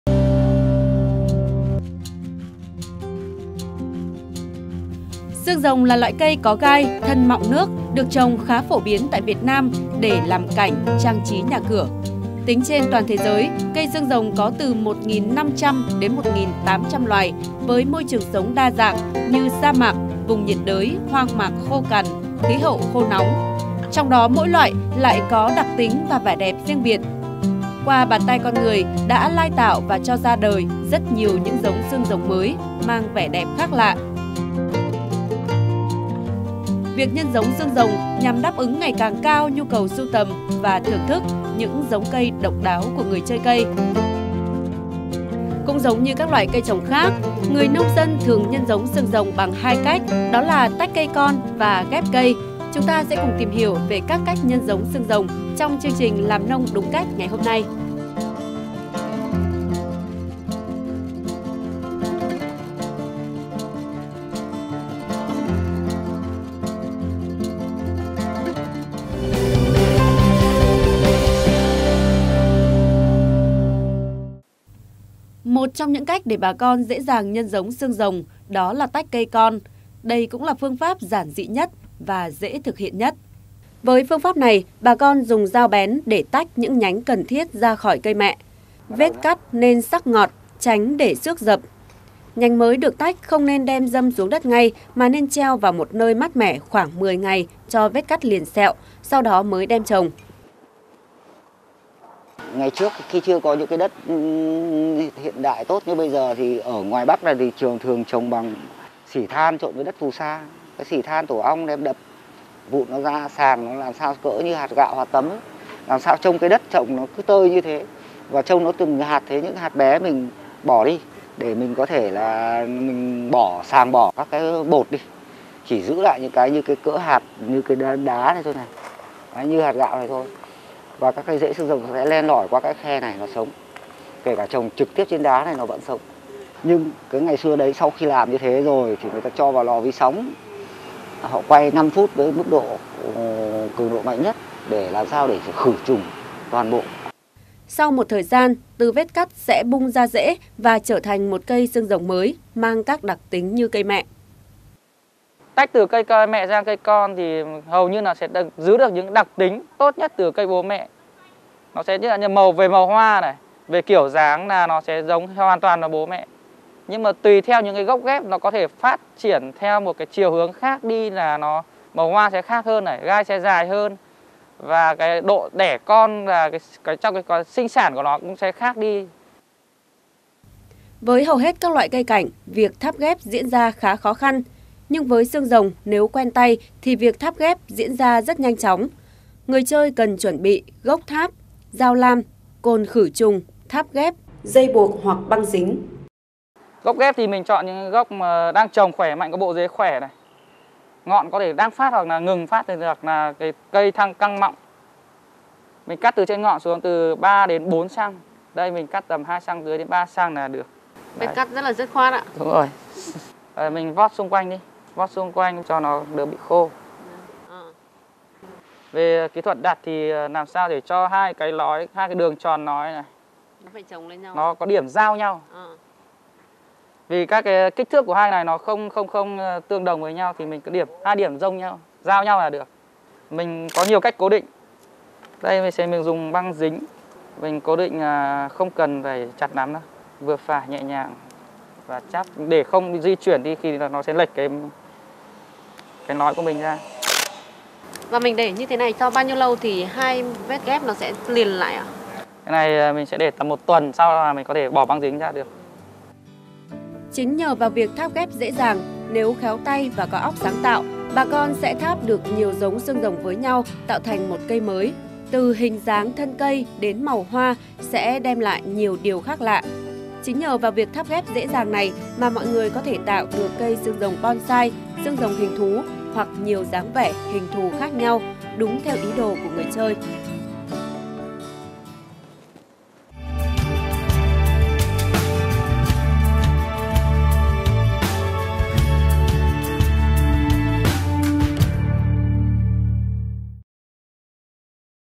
Bí quyết nhân giống xương rồng | VTC16
VTC16 | Nhân giống xương rồng có 2 cách: tách cây và ghép cây. Qúa trình nhân giống xương rồng có gì đáng lưu ý?